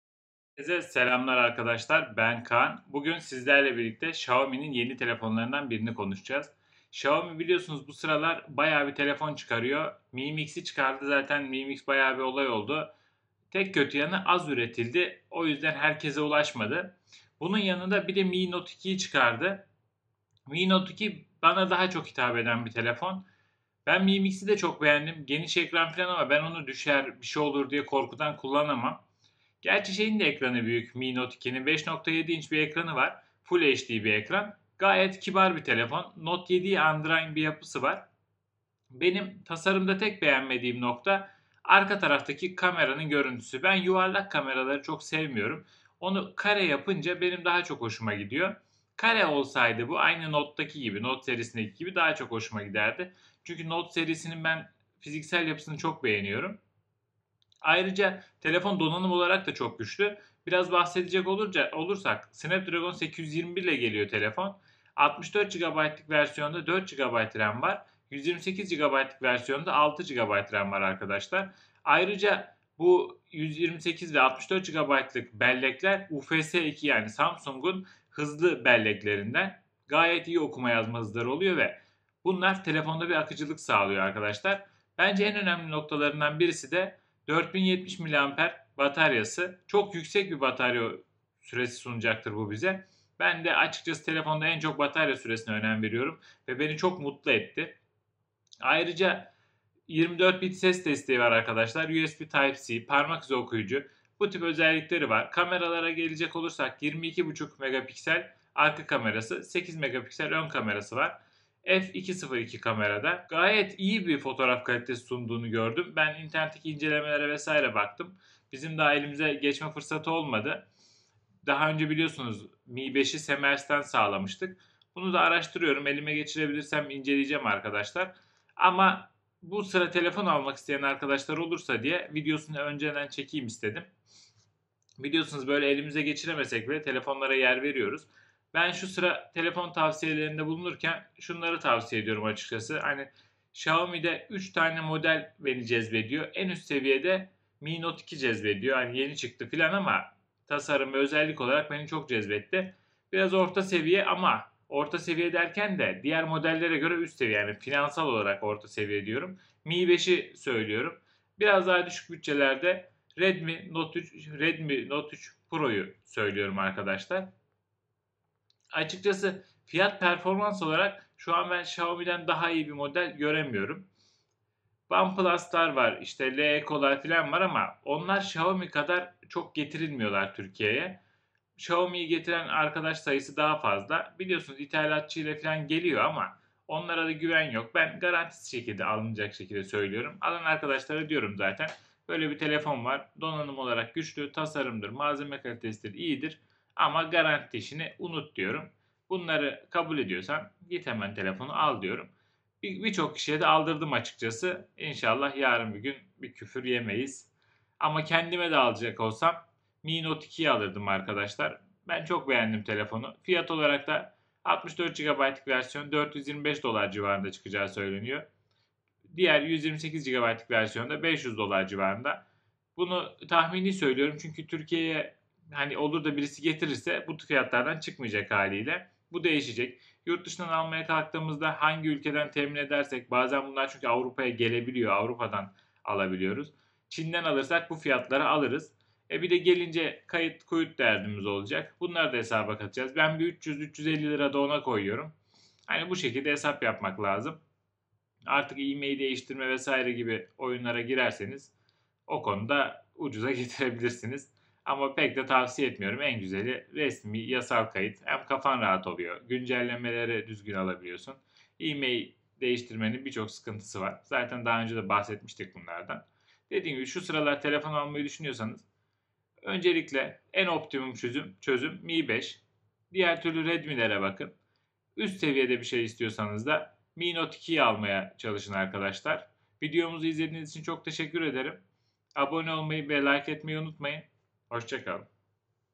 Xiaomi Mi Note 2 Değerlendirmesi
Herkese merhaba sevgili Scroll izleyenleri, bu videomuzda piyasaya yakın zamanda sürülmesi beklenen ve Xiaomi firmasının performans ürünlerinden biri olan Mi Note 2 modelini sizler için değerlendirdik. Keyifli seyirler :)